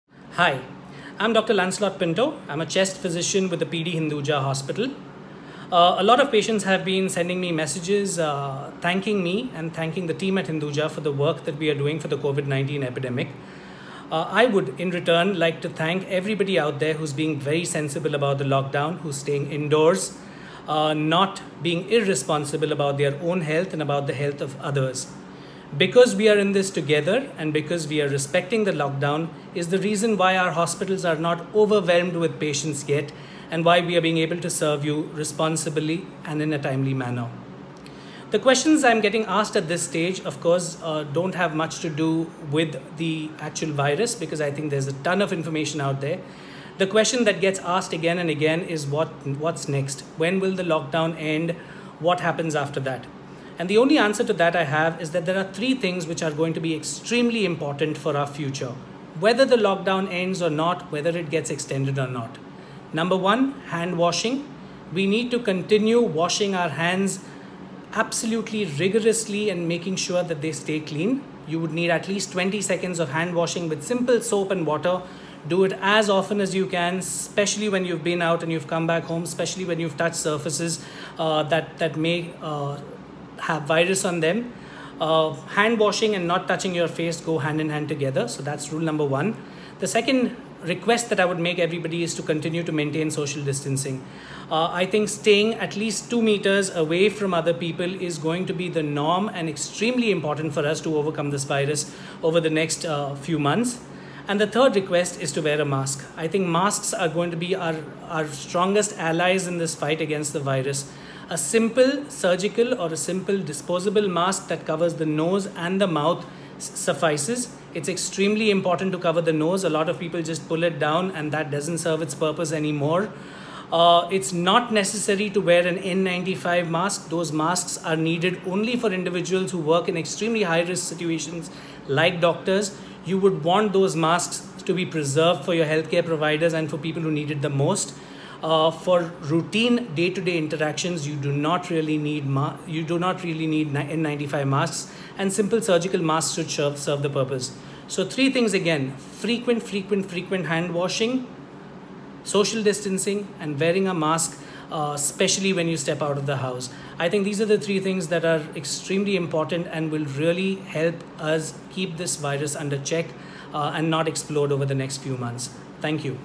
Simple steps to curb the spread of COVID-19
Dr. Lancelot Pinto, Consultant Respirologist at P. D. Hinduja Hospital shares simple tips to help curb the spread of COVID-19.